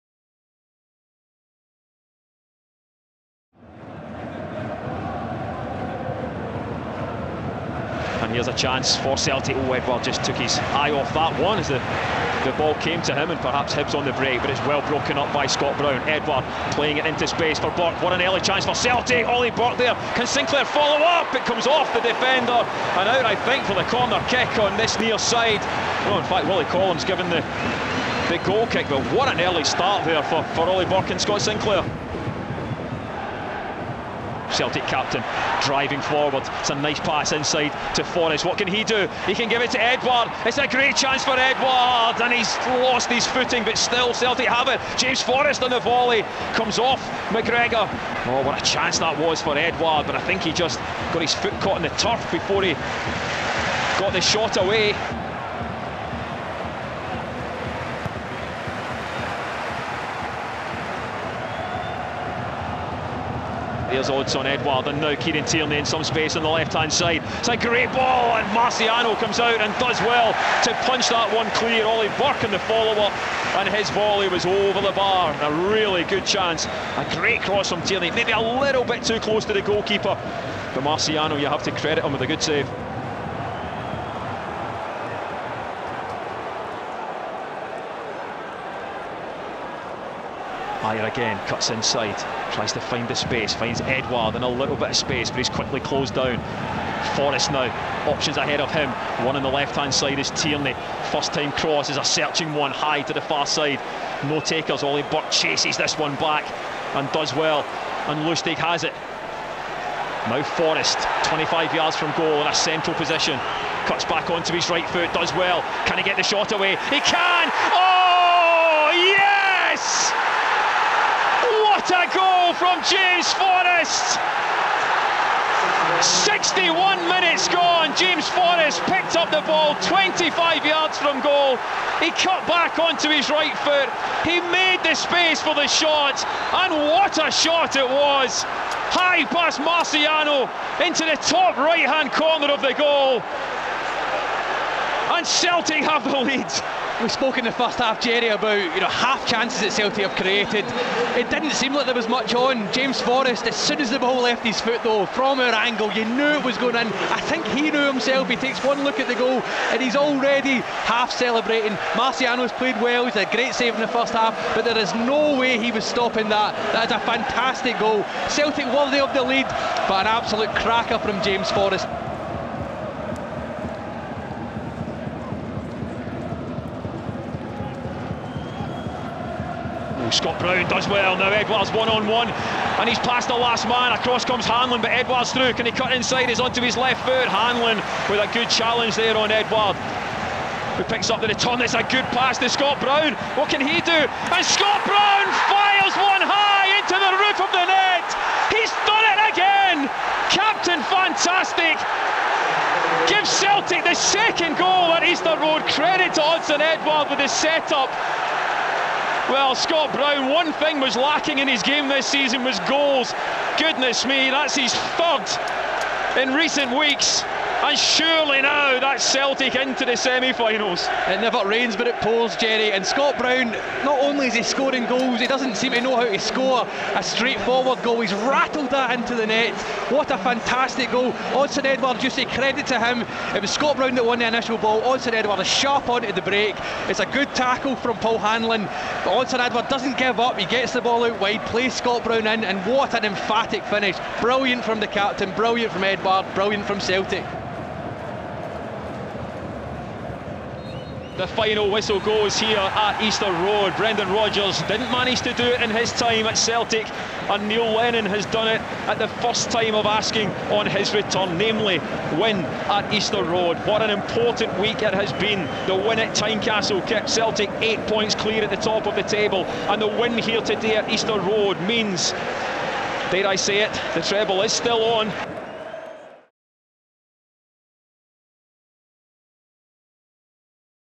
Hibernian 0-2 Celtic | Belters from Forrest and Brown send Neil Lennon's Celts to Semi-finals!
Watch Match Highlights as as James Forrest and Scott Brown ensure the Bhoys book their place in the semi-finals of the Scottish Cup.